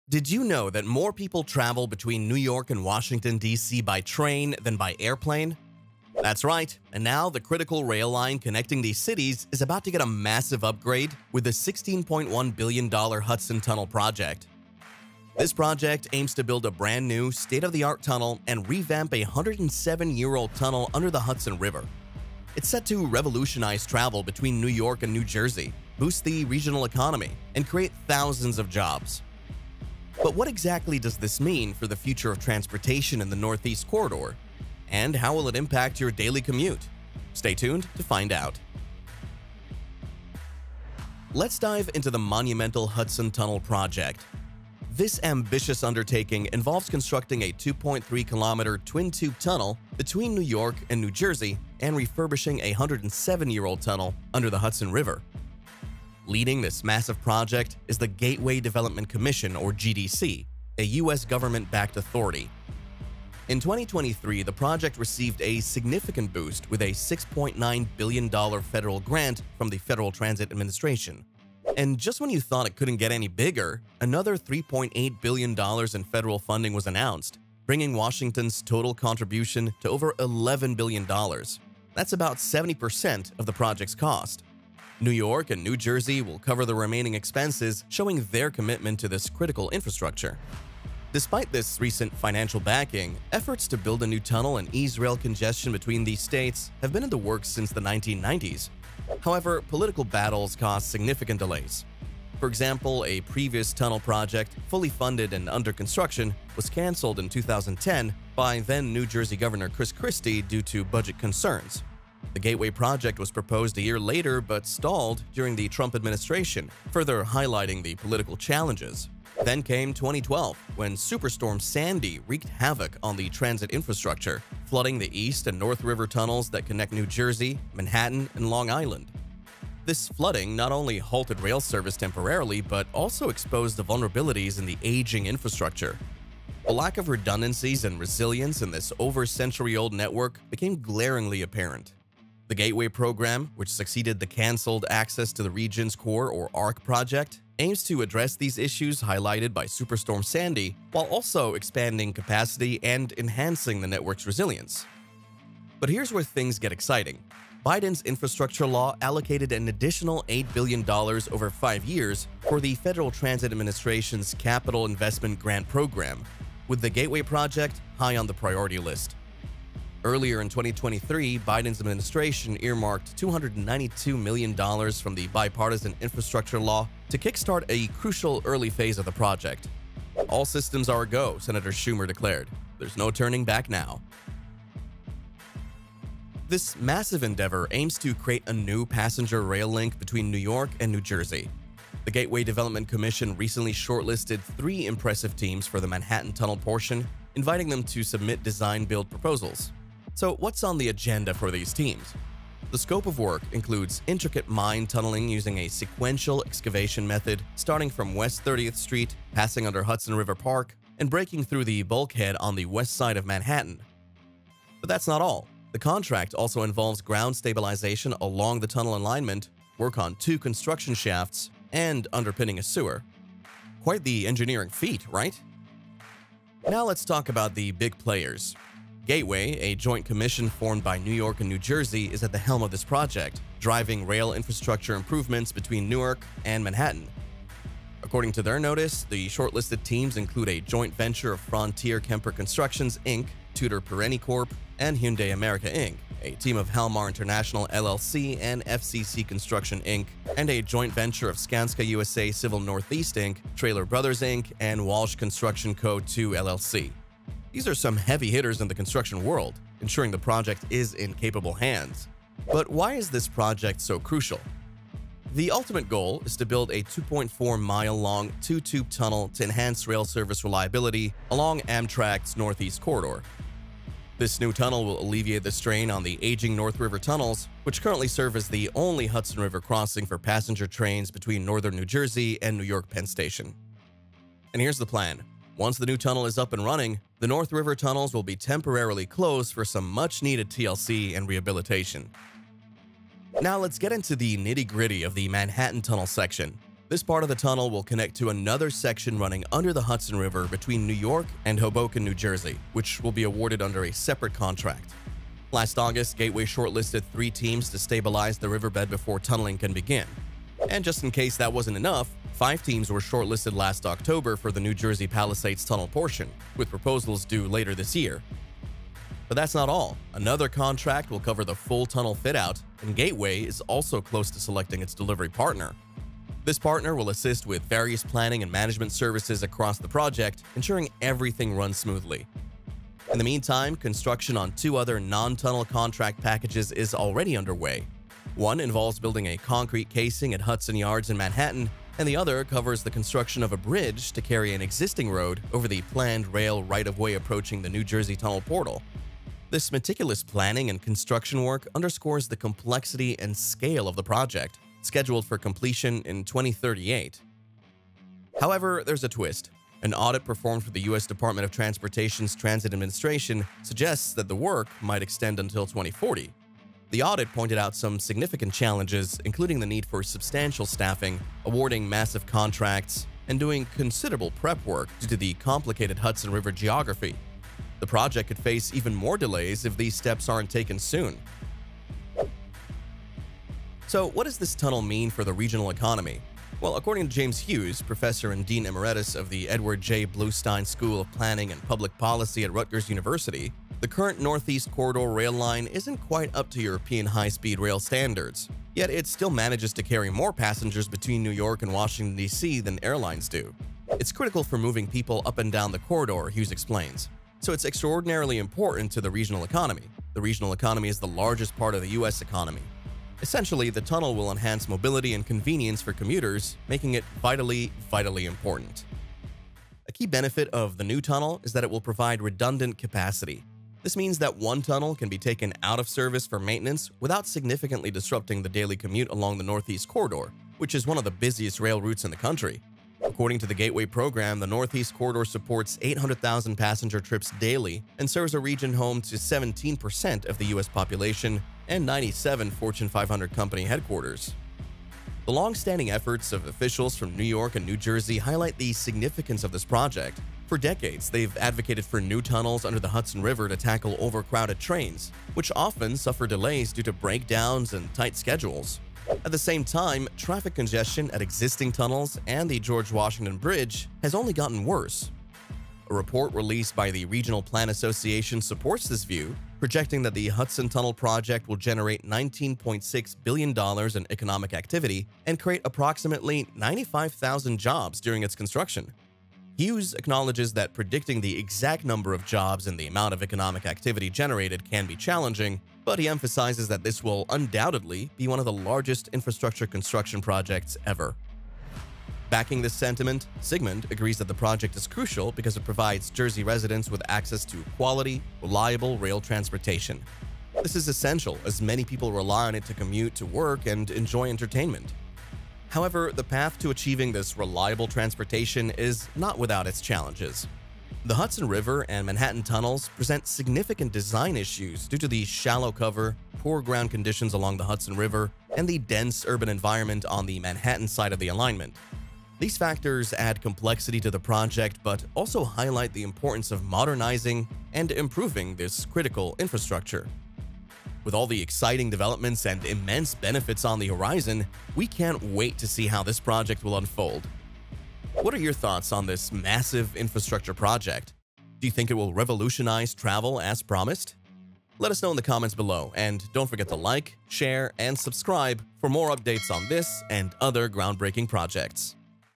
Exploring New York's $16 Billion Mega-railway Project Beneath The Hudson River
Inside New York’s New $16B Mega-Railway Right Under the Hudson River

Let's dive into the monumental Hudson Tunnel project! This ambitious undertaking involves constructing a 2.3km twin-tube tunnel between New York and New Jersey and refurbishing a 107-year-old tunnel under the Hudson River.

00:00 Introduction
00:40 The Hudson River Tunnel Project
03:05 Engineering Marvel
06:23 Economic Impact to NYC